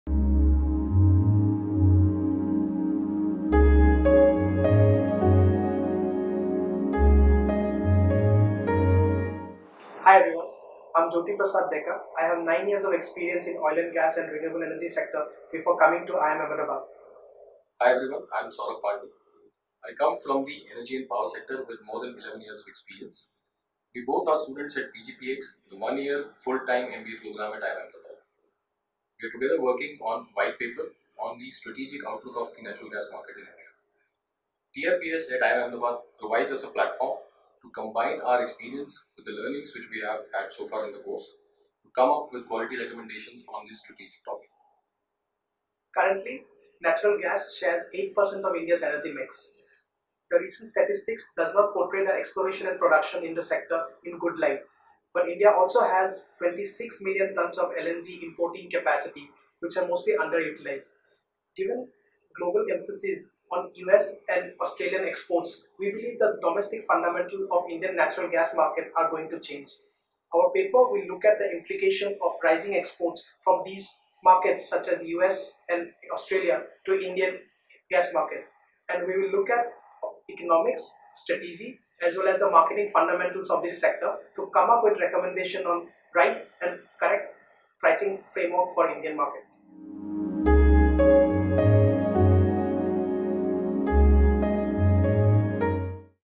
Should India look at exploring its own sources of natural gas?
Indian natural gas sector is on the cusp of fundamental change due to rise in import based supply.

Will exports from new sources such as the United States and Australia make the price of natural gas affordable for developing economies like India?

Will the recent developments in the world natural gas market compel policy makers in India to liberalize the domestic market and bring reforms in pricing?

The white paper, by IIMA PGPX students Jyoti Prasad Deka, Prashasti Saxena, Saurabh Pandey and Sudhanshu Prasad evaluates these raging questions and tries to arrive at probable answers.

Join us on 29th and 30th September to know more about this through The Red Brick Summit Connexions.